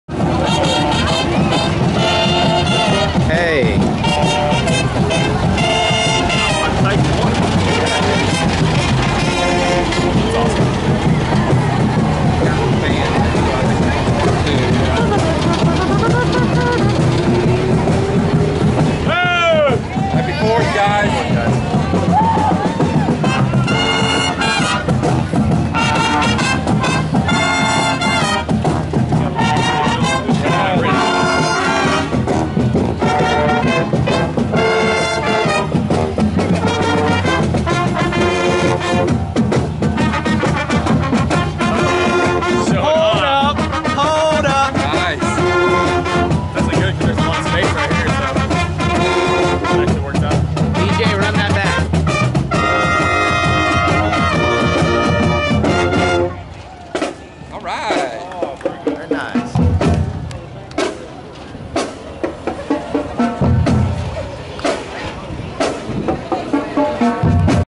The Union Pacific Mini-Train
The UP Mini-train visits TheWoodlands 4th of July Parade 2015.